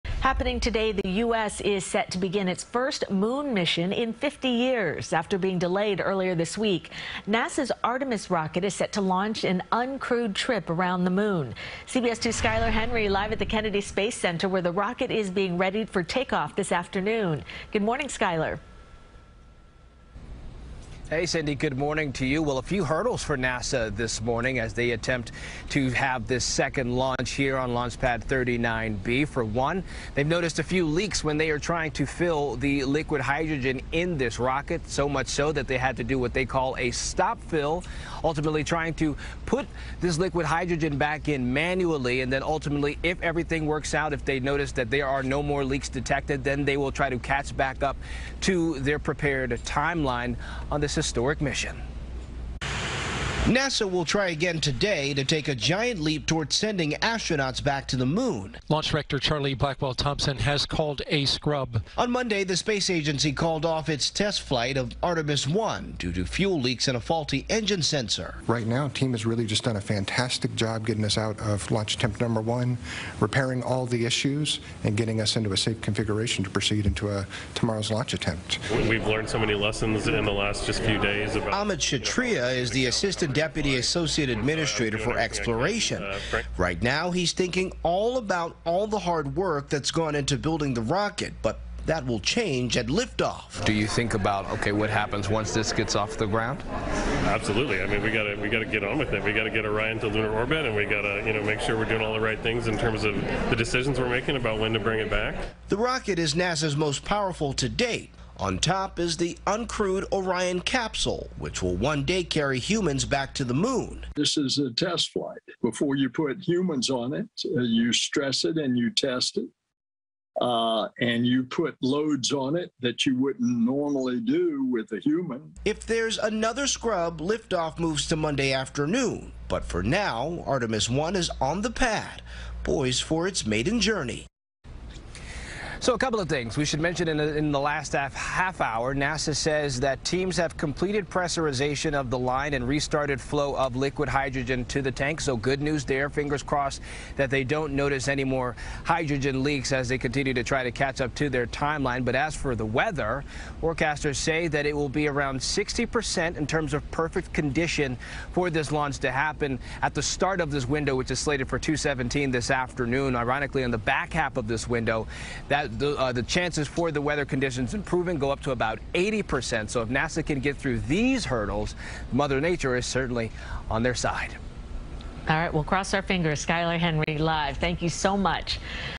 NASA ready for second Artemis launch attempt
NASA's first moon mission in 50 years was delayed earlier in the week because of a fuel leak and faulty sensor, CBS2's Skyler Henry reports.